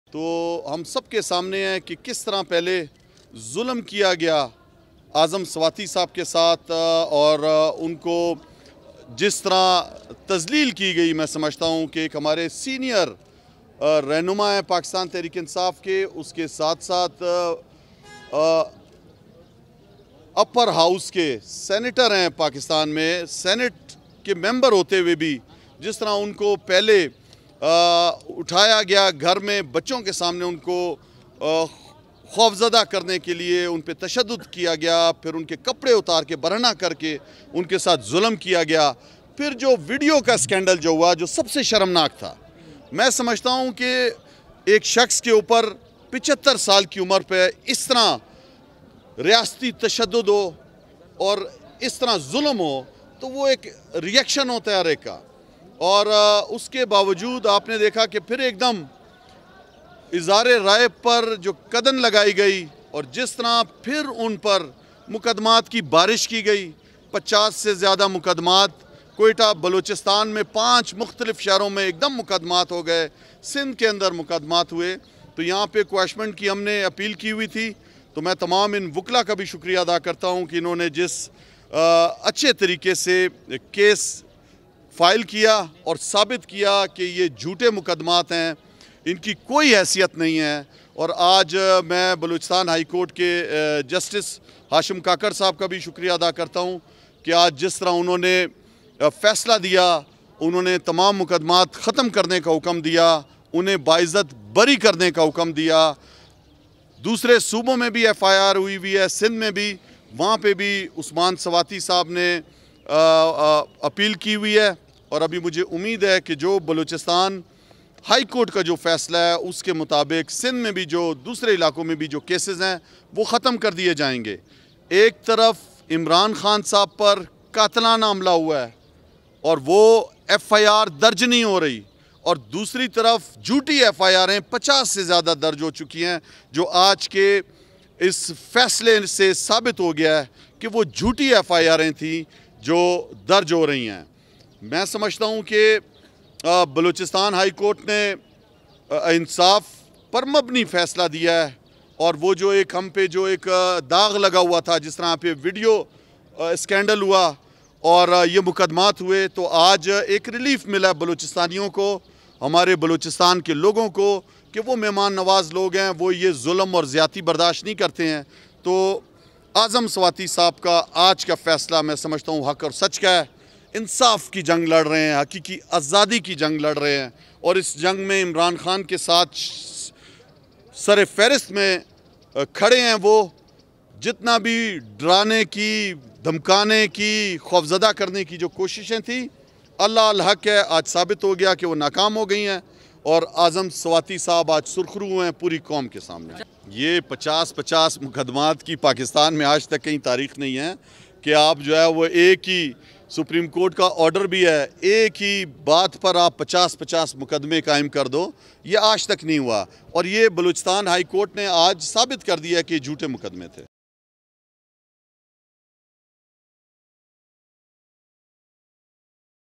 Senator Azam Swati gets major relief from Balochistan High Court, orders release
سینیٹر اعظم سواتی کو بلوچستان ہائیکورٹ سے بڑا ریلیف مل گیا، رہائی کے احکامات